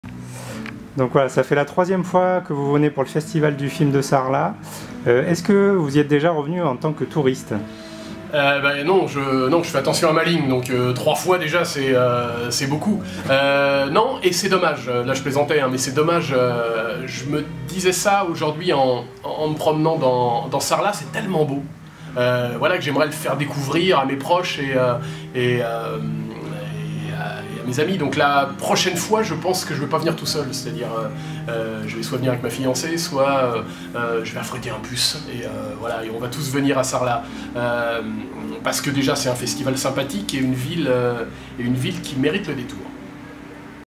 Le réalisateur Fred Cavayé prend un abonnement pour Sarlat
Le réalisateur de "Adieu Monsieur Haffmann" est une habitué du Festival du film de Sarlat pour y être venu trois fois. Il adore la manifestation et la ville qu'il souhaite déguster en revenant en touriste.